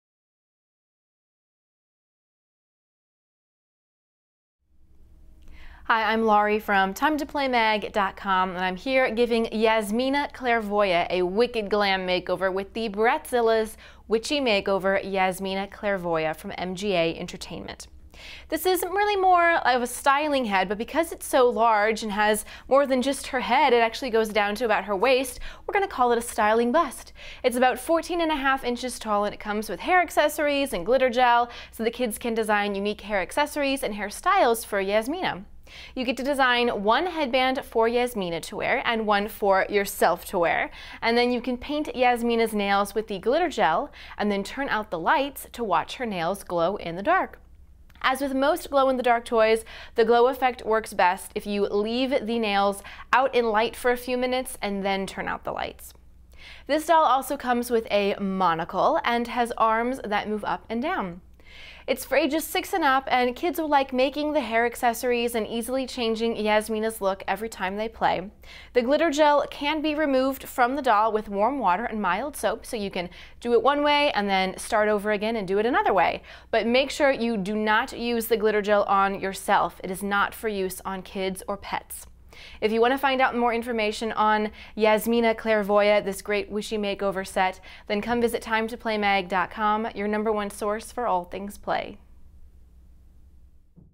Bratzillaz Witchy Makeover Yasmina Clairvoya from MGA Entertainment
See the full review, the updated price and where it's available to buy

Kids can give Yasmina Clairvoya a wicked glam makeover with the Bratzillaz Witchy Makeover Yasmina Clairvoya. This 14.5-inch styling bust comes with a hair bow, a hair net, a top hat, a hair brush, glitter gel, two hair clips, and two headbands. Using these supplies, kids can design unique hair accessories and hairstyles for Yasmina. Kids can design one headband for Yasmina and one for themselves. Paint Yasmina's nails with the glitter gel, then turn out the lights to watch the nails glow in the dark. The doll also comes with a monocle. Yasmina's arms also move up and down.